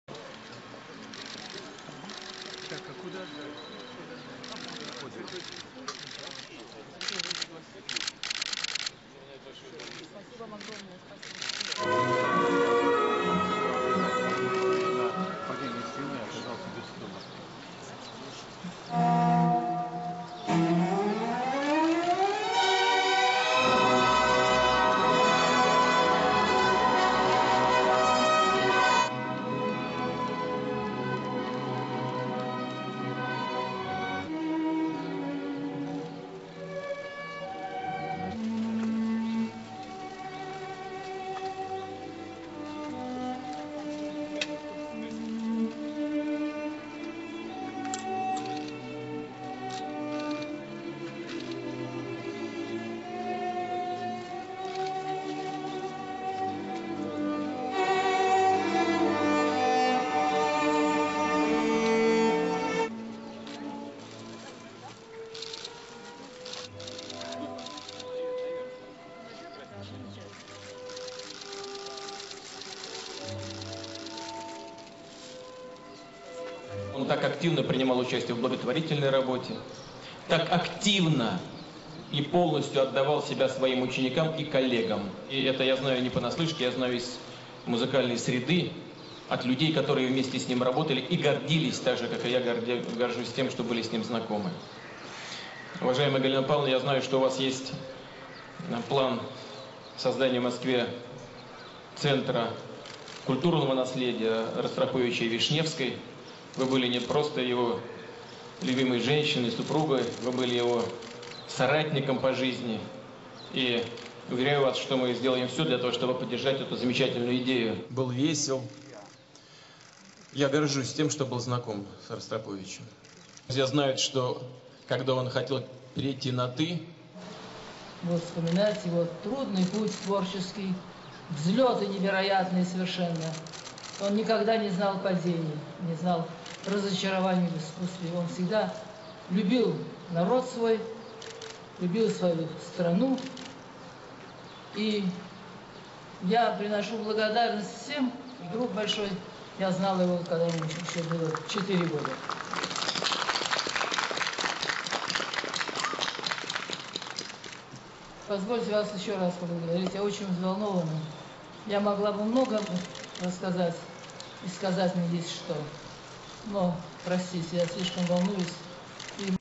Mar 29, 2012 Russia_Putin unveils statue of Russian musician Rostropovich
Russian Prime Minister Vladimir Putin attended the unveiling ceremony of a statue of the late Soviet and Russian cellist and conductor Mstislav Rostropovich, on Thursday.

The statue, showing Rostropovich playing the cello, is located in the center of Moscow, where he lived with his wife, singer Galina Vishnevskaya.

The unveiling of the statue was the main event of the 3rd International Festival of Mstislav Rostropovich, which began on March 27, the birthday of the musician who died in 2007.

Putin promised Rostropovich's widow Galina Vishnevskaya that he would support the establishment of the Center of Cultural Heritage of Rostropovich in Moscow and said he is proud to have been acquainted with the famous musician.

According to the prime minister, the magnificent statue will "certainly beautify the capital."

Vishnevskaya thanked all who came "to honor the memory of the great citizen of Russia", and also recalled that Rostropovich had a difficult career, but "he did not accept any failures or disappointments in his art".

After the ceremony, Putin and other guests laid flowers at the statue.